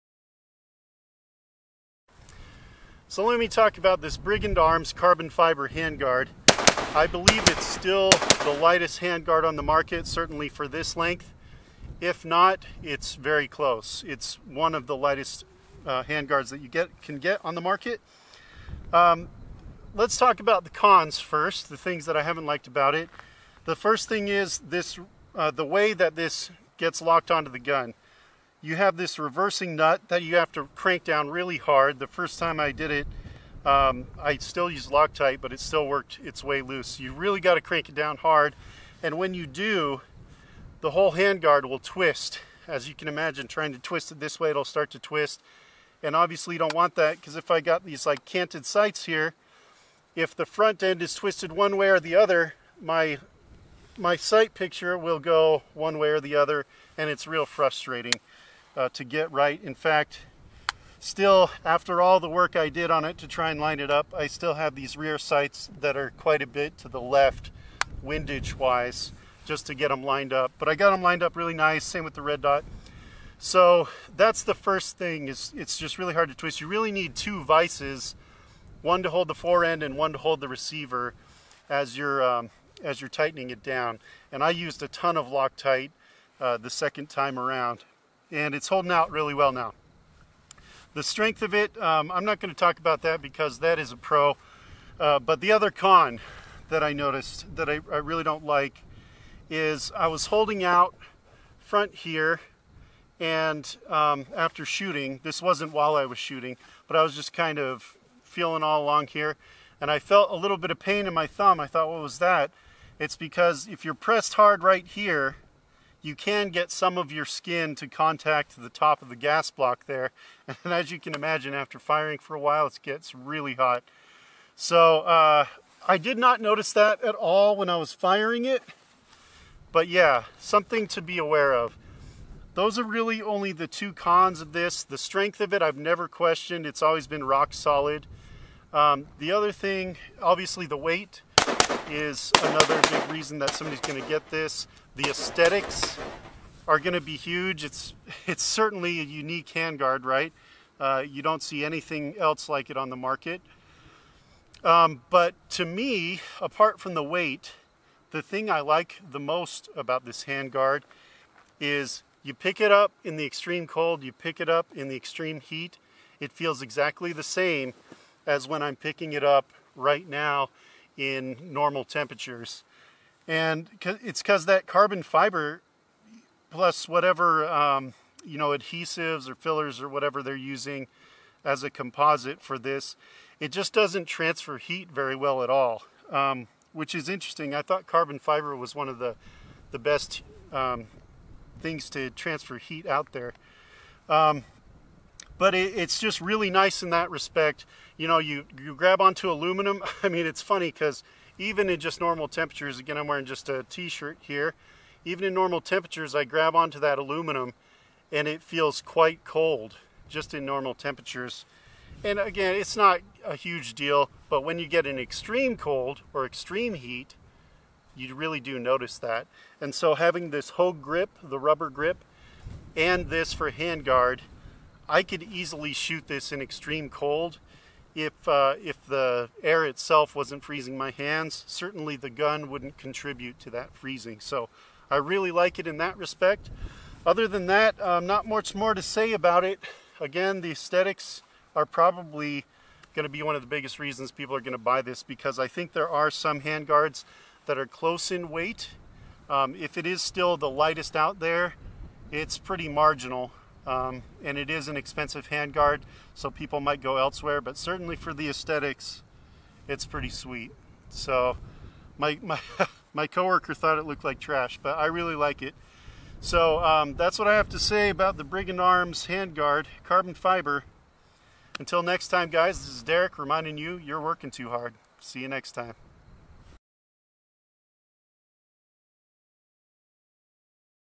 Brigand Arms Carbon Fiber Handguard (Atlas) Review
My thoughts on the lightest AR15 handguard on the market. Made from woven carbon fiber! Woo Hoo, space age!